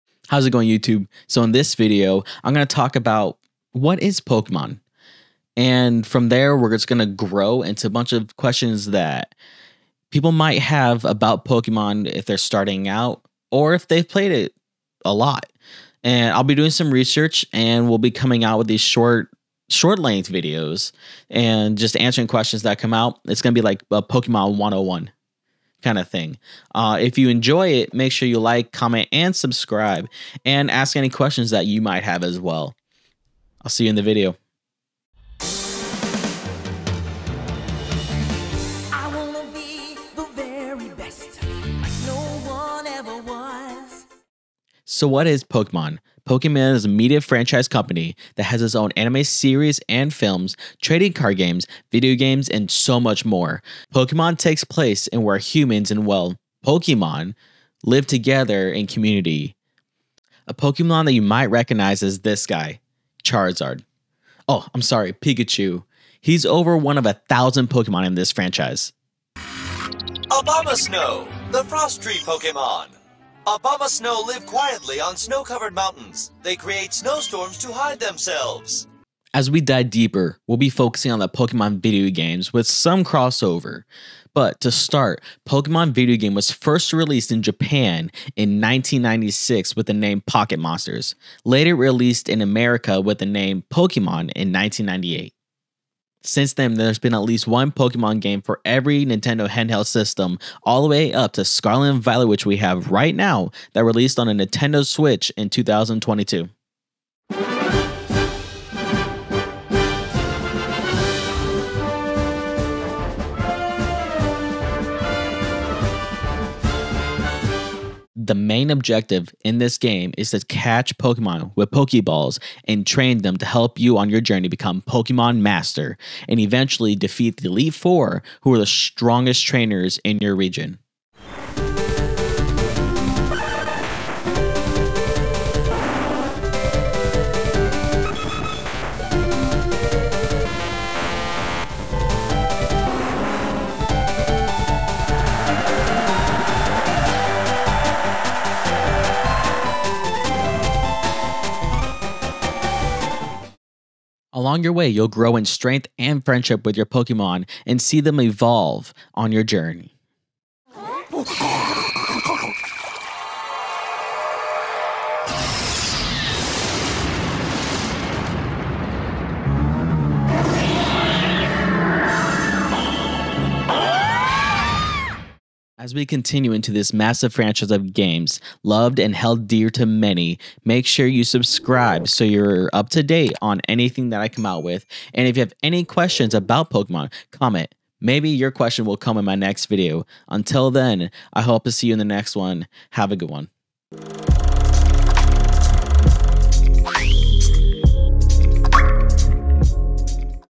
What Is Pokemon?
Going through a basic explanation on what is Pokemon and how it has changed throughout the years